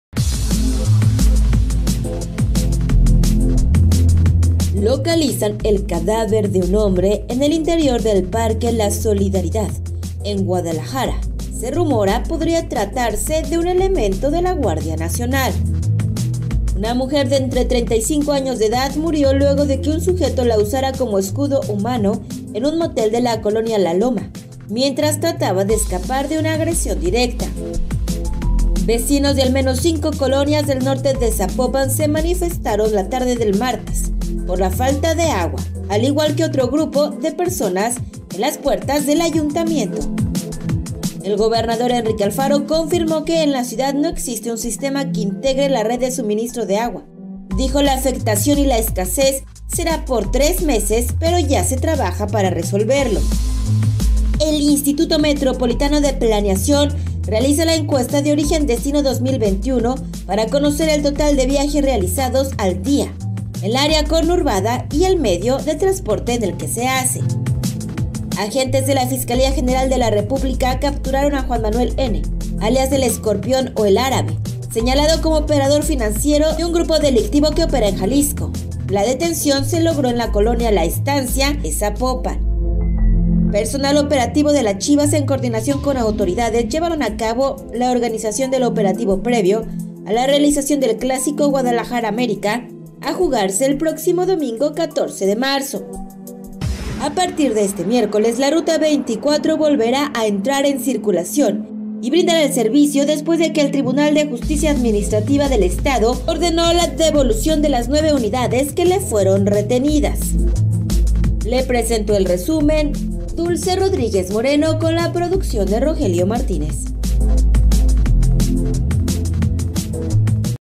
Falta de agua en ZMG durará 3 meses | Regresa la ruta 24 | Alistan operativo para el clásico
RESUMEN ZONA 3 09 MARZO 21